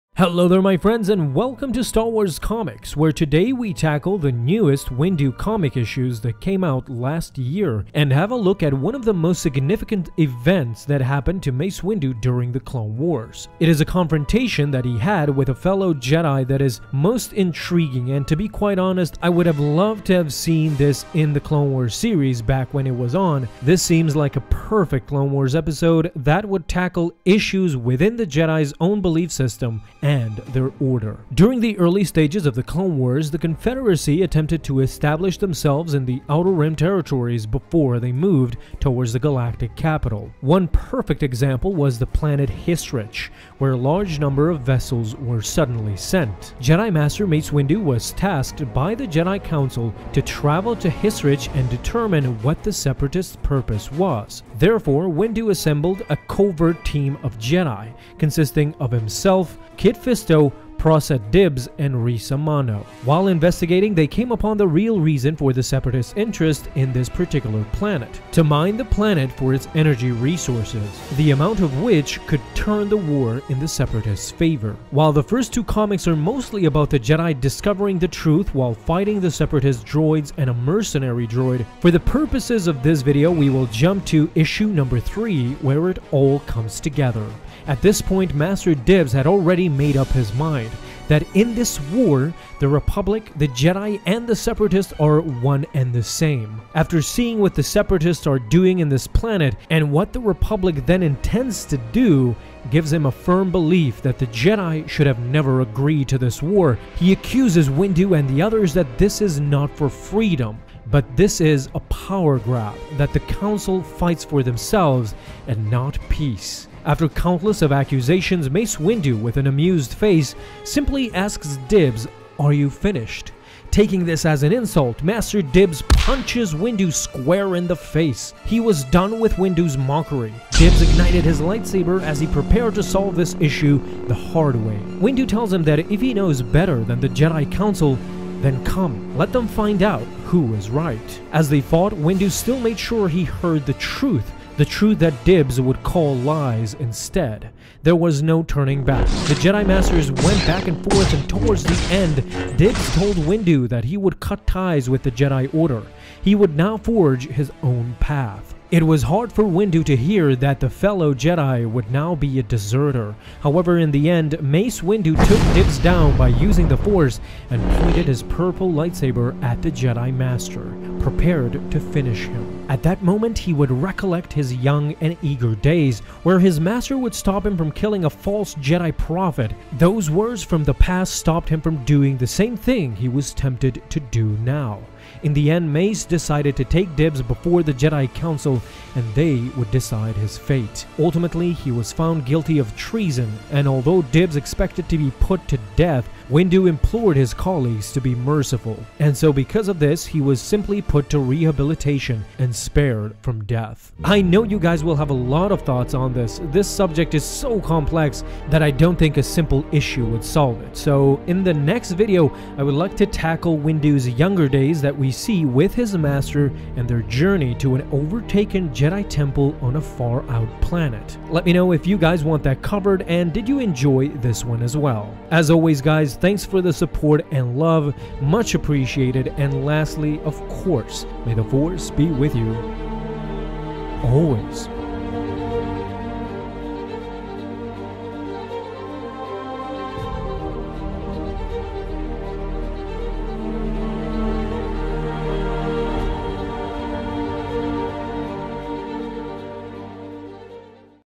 Why Mace Windu Was PUNCHED By Another Jedi(CANON) - Star Wars Comics Explained
After the Clone Wars had just started Master Mace Windu was sent on a mission to an Outer Rim Planet, while there a scuffle ensued between him and another Jedi Master.

Source:  Mace Windu - Jedi of the Republic #3-4